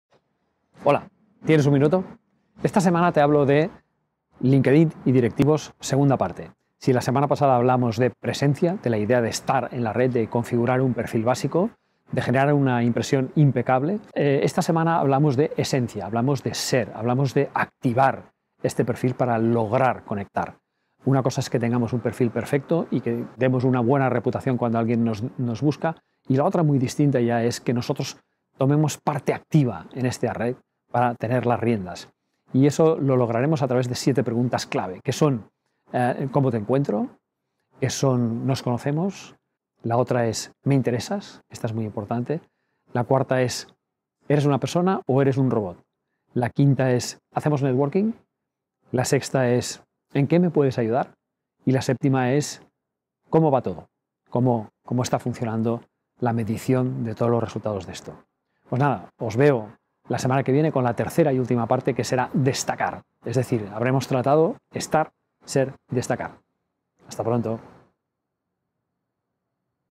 El manager y LinkedIn parte 2 de 3 SER Activar el perfil
Esta semana te hablo de LinkedIn y directivos, 2ª parte.

Si la semana pasada hablábamos de presencia, de la idea de ESTAR en la red, de configurar un perfil básico, de generar una impresión impecable, esta semana hablamos de esencia, hablamos de SER, hablamos de activar este perfil para lograr conectar.

Una cosa es que tengamos un perfil perfecto y que demos una buena reputación cuando alguien nos busca, y la otra muy distinta es que nosotros tomemos parte activa en esta red para tener las riendas.

Y eso lo lograremos a través de siete preguntas clave, que son: ¿Cómo te encuentro? que son ¿Nos conocemos? La otra es ¿Me interesas? Esta es muy importante. La cuarta es ¿Eres una persona o eres un robot? La quinta es ¿Hacemos Networking? La sexta es ¿En qué me puedes ayudar? Y la séptima es ¿Cómo va todo? Cómo está funcionando la medición de todos los resultados de esto.